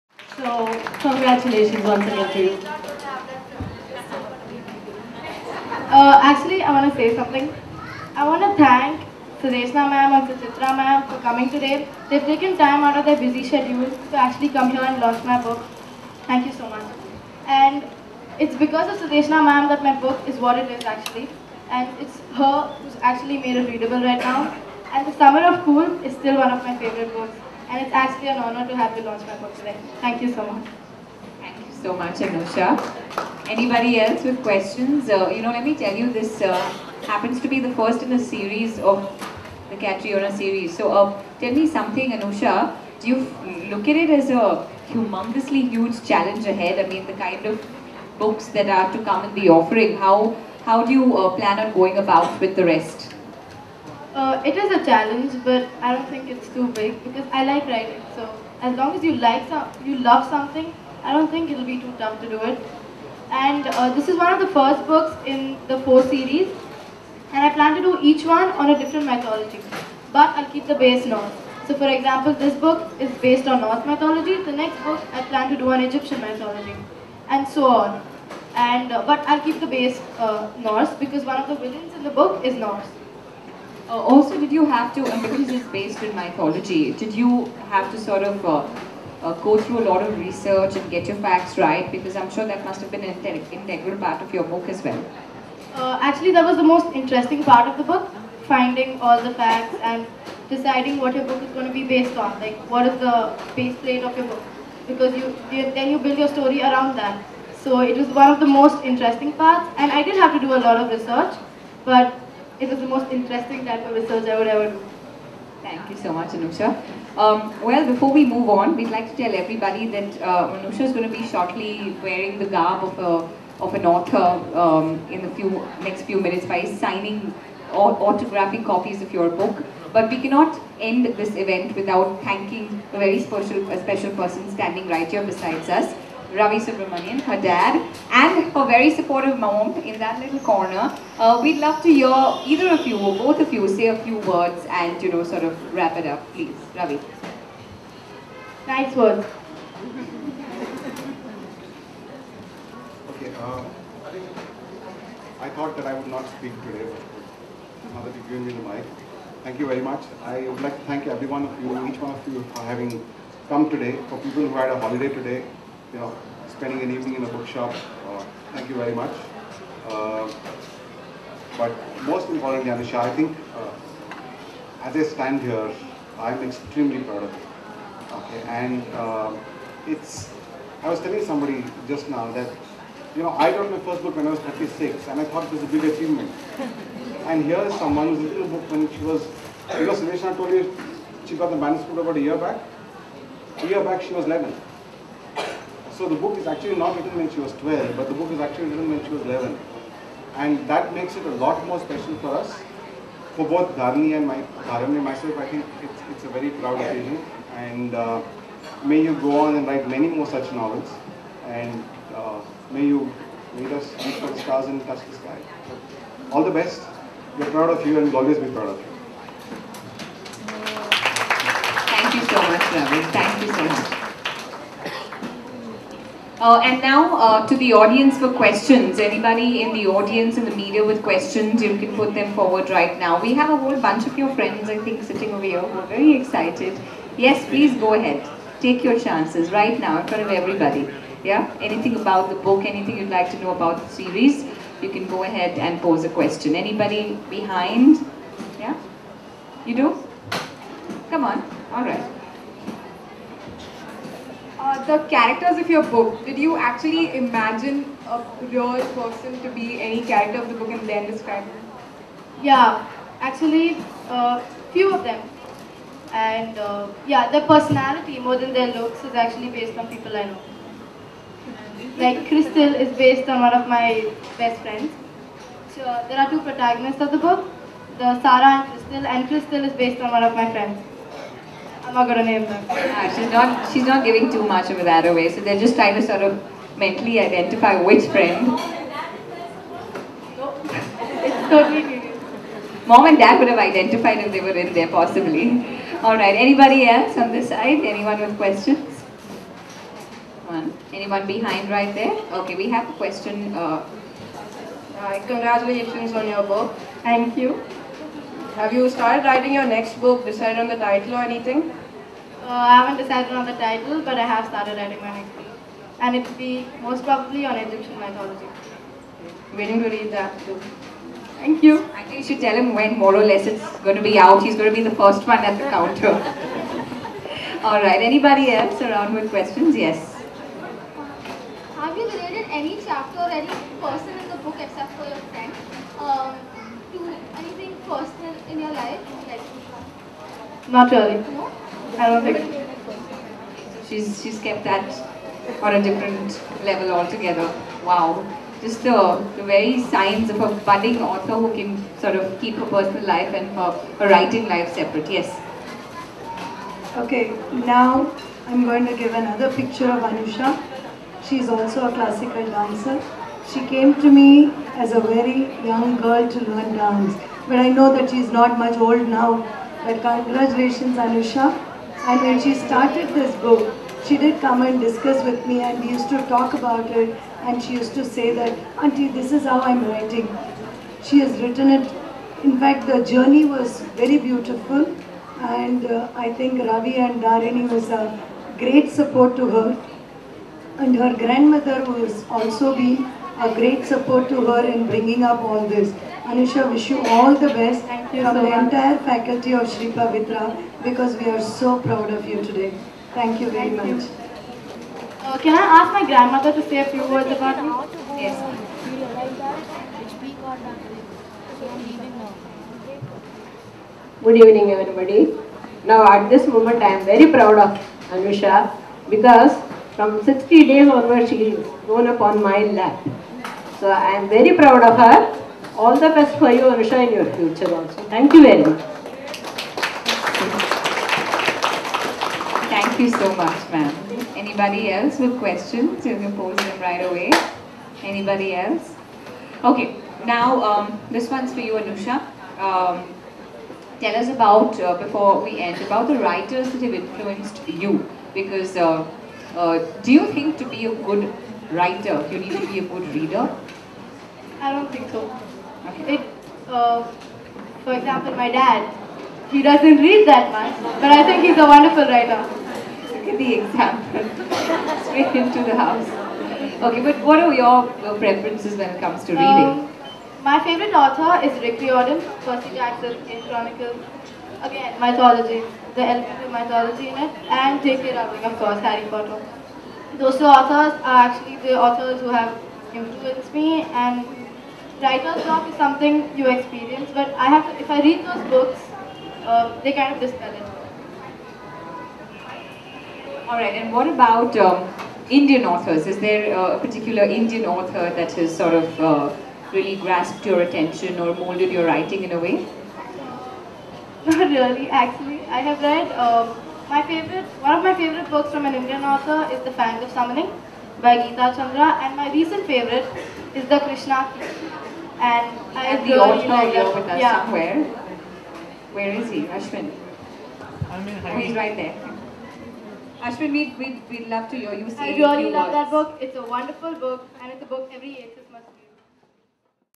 Book Launch of Heirs of Catriona By Anusha Subramanian - 3
One of India's youngest published authors, Anusha Subramanian, debuts with her fantastic, action-packed novel.

In a land like no other on earth, deadly creatures from forgotten myths are coming alive...

Sara and Crystal are ordinary teenagers. Or so they think, until one day, their world is turned upside down when they learn that they are princesses of the magical land of Catriona. Their mothers, the rightful rulers of Catriona, are being held prisoner by Merissa, the wicked queen. Sara and Crystal must help rescue their mothers, and to do this, they have to journey to a land that does not appear on any known map. Thrown headlong into a world of vengeful, manipulative gods and deadly sorcerers, the two girls learn the art of magical warfare. But will that be enough to counter the evil unleashed by Merissa and her protector, Loki?

Filled with fantastic action, adventure and intrigue, Heirs of Catriona teenage author Anusha Subramanian's first book in the Catriona series casts a spell on the reader from the very first page.

Anusha Subramanian
One of the countrys youngest published authors, Anusha Subramanian was only twelve when she wrote Heirs of Catriona. A voracious reader, Anusha is a student of seventh grade at a leading school in Mumbai.